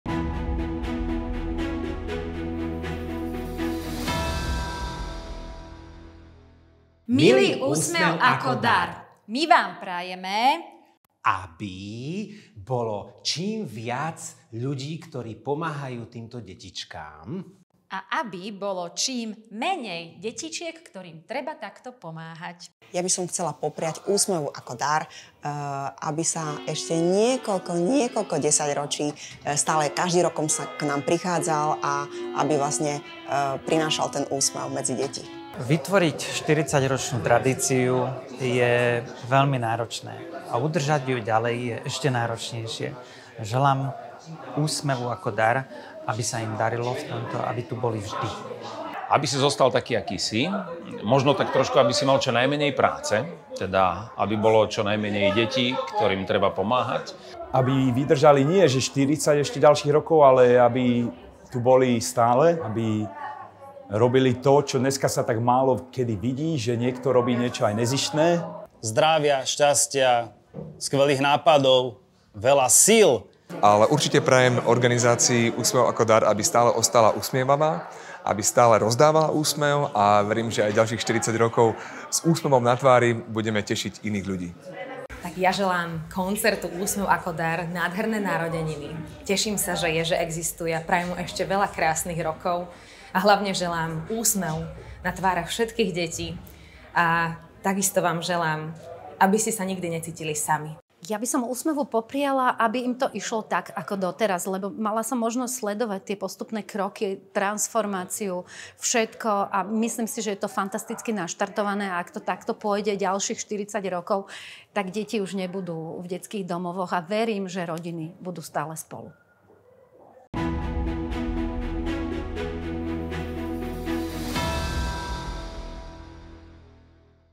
Priane umelcov k 40. narodeninám Úsmevu ako dar
Pozrite si čo nám popriali umelci, ako Mária Čírová, Ego, Laci Strike, Spievankovo a aj mnoho daľších, k našim 40. narodeninám.